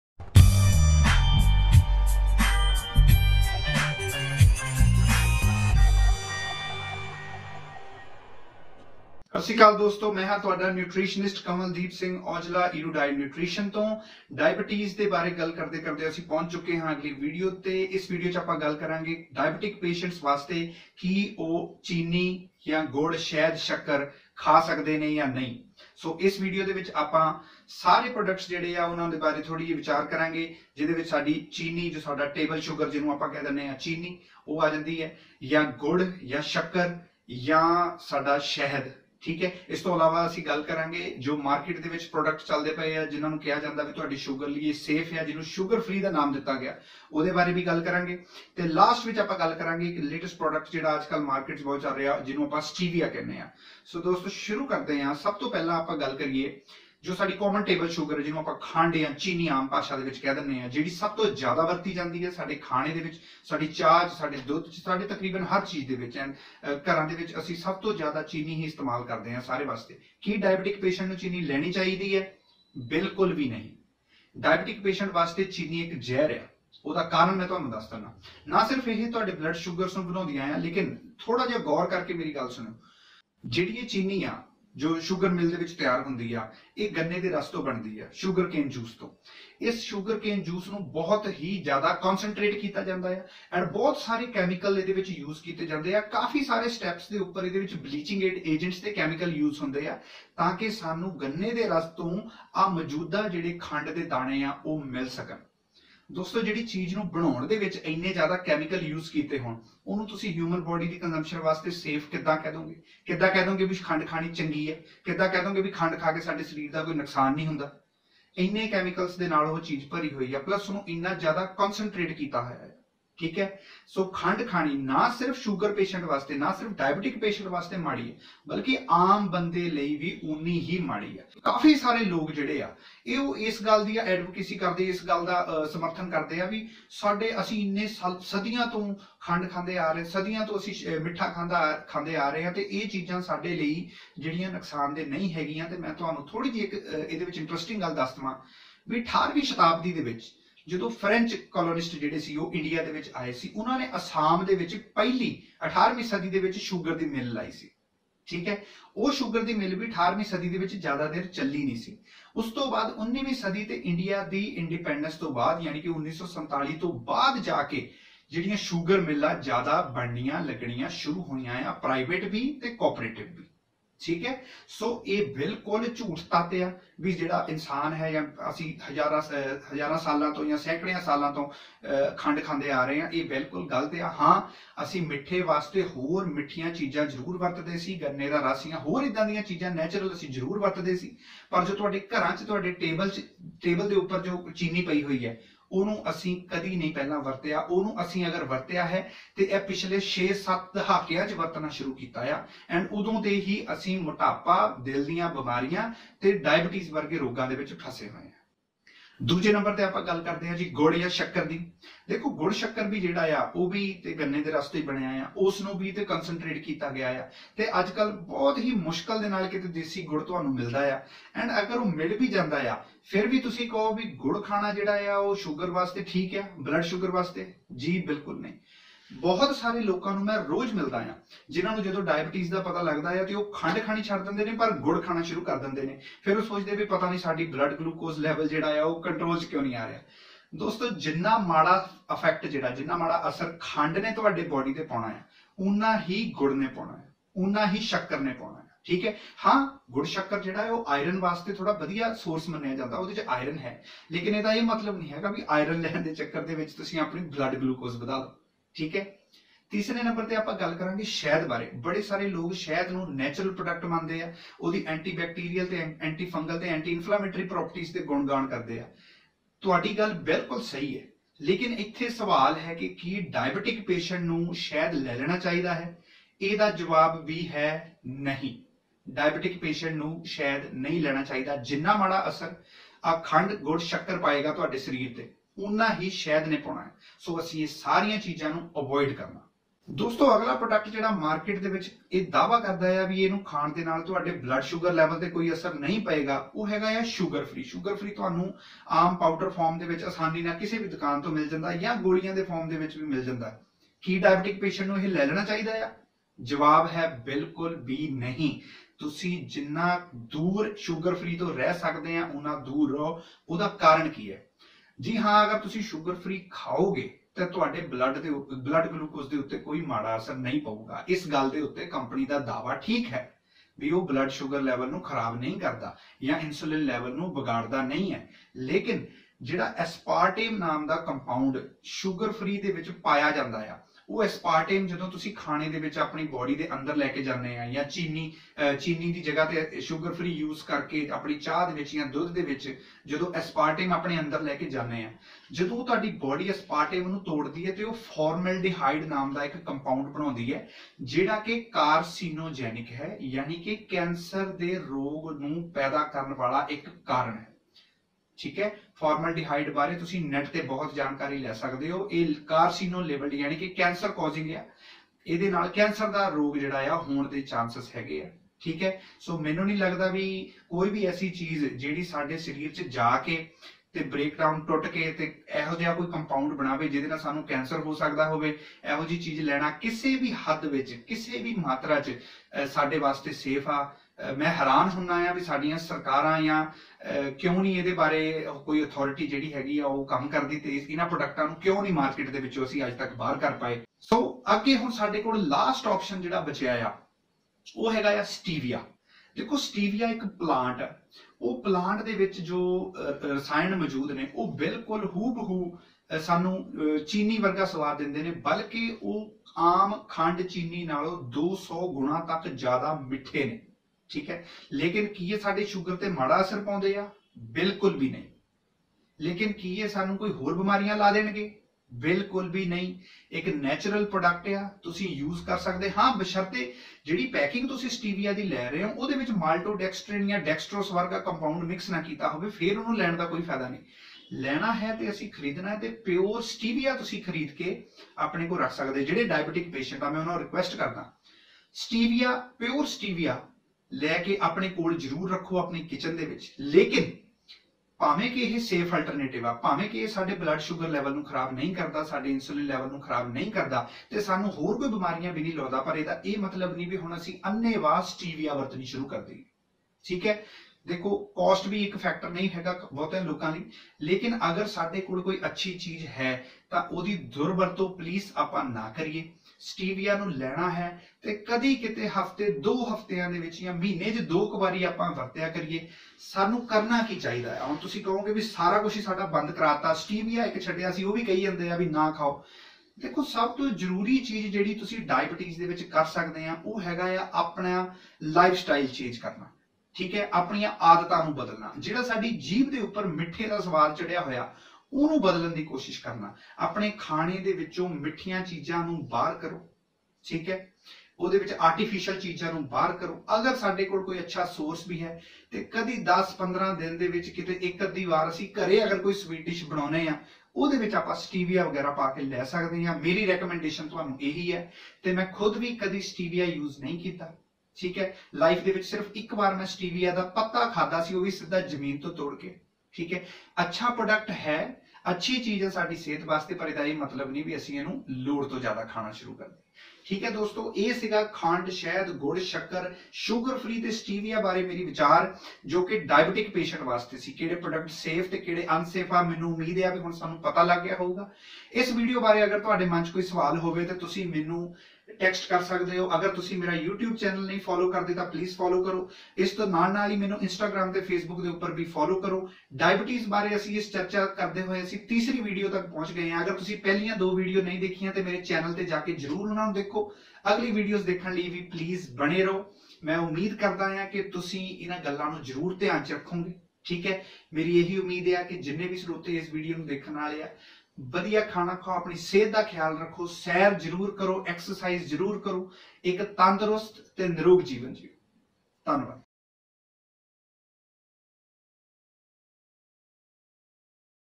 CAN DIABETICS EAT SUGAR OR SUBSTITUTES? BY Erudite Nutrition (PUNJABI)
in this video I discuss the sugar products and sugar substitutes which are popular in India. we will also clarify some doubts, and affects of such products on blood glucose levels.

KANWALDEEP SINGH AUJLA
NUTRITIONIST & DIETETICS COUNCILLOR
Erudite Nutrition - Definition of Nutrition